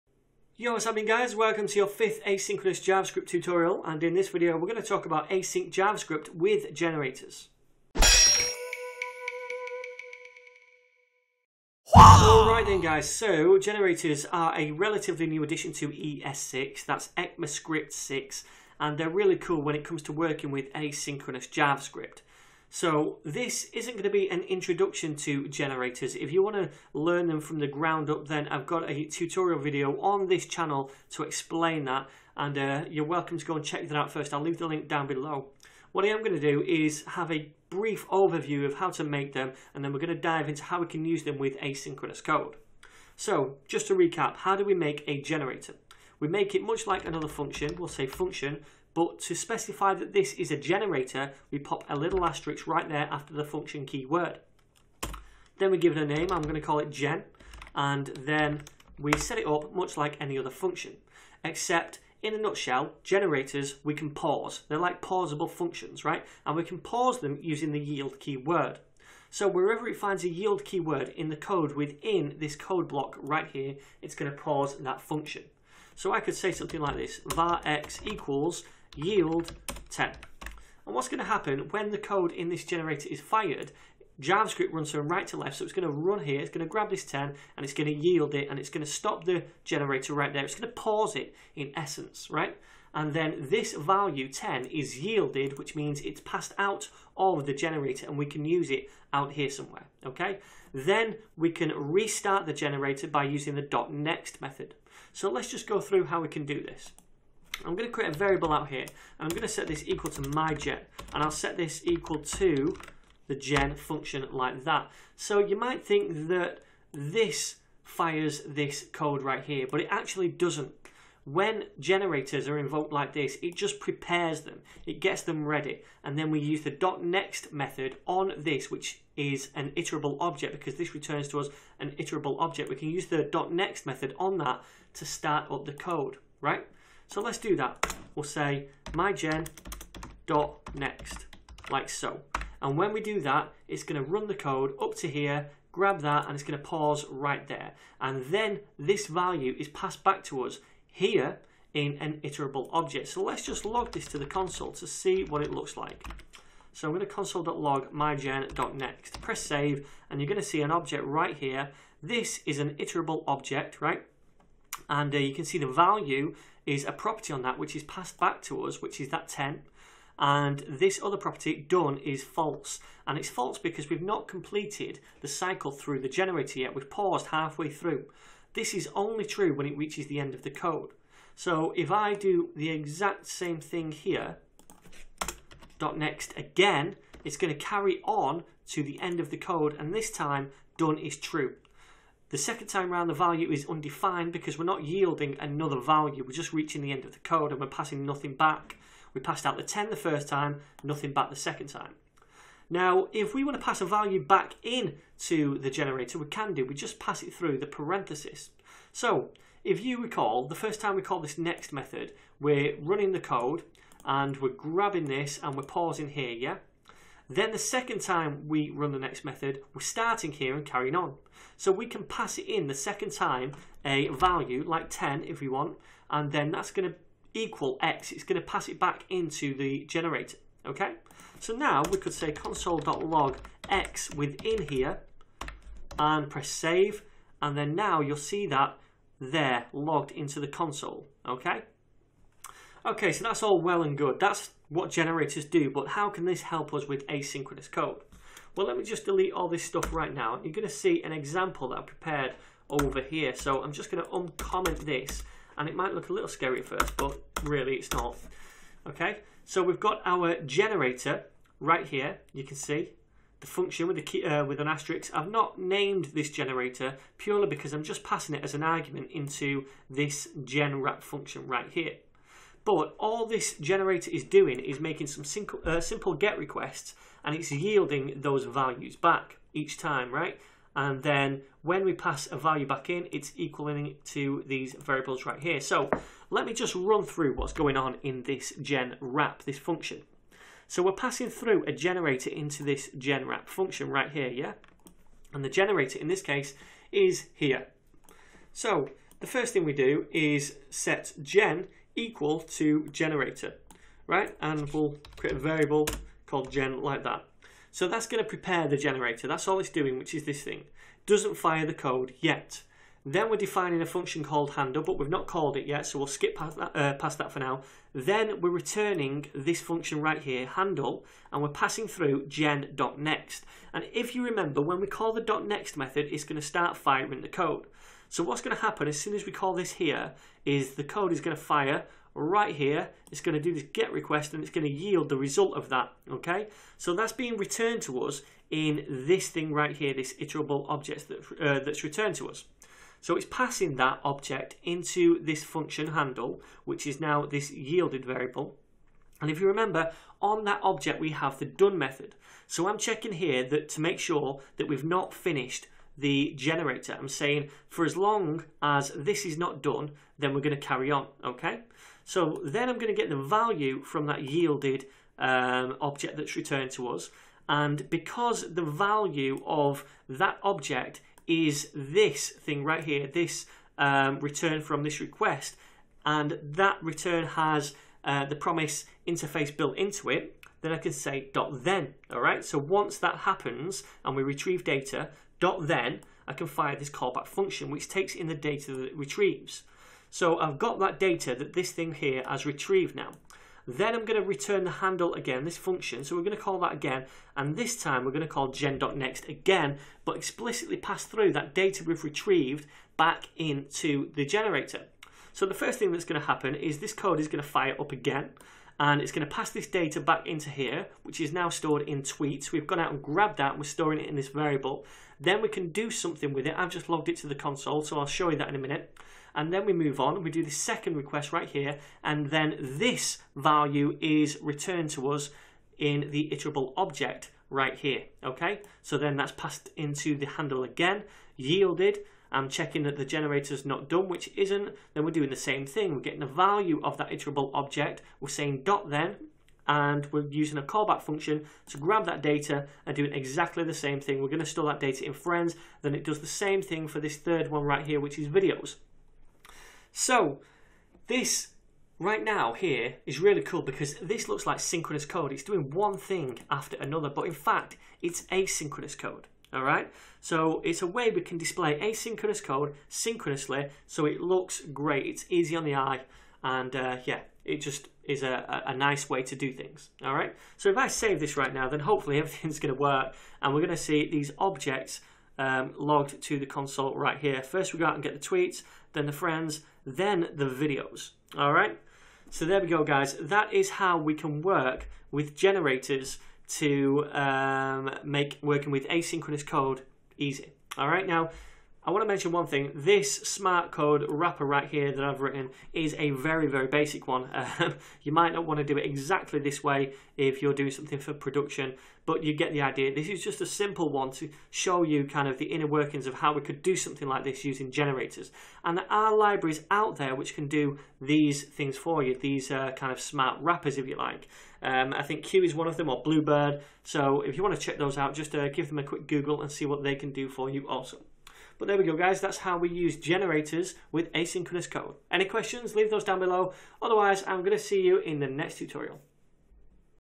Asynchronous JavaScript #5 - Generators
Hey gang, in this tutorial, I'll show you how we can set up generators to process our asynchronous JavaScript code, and make it look like synchronous code!

Social Links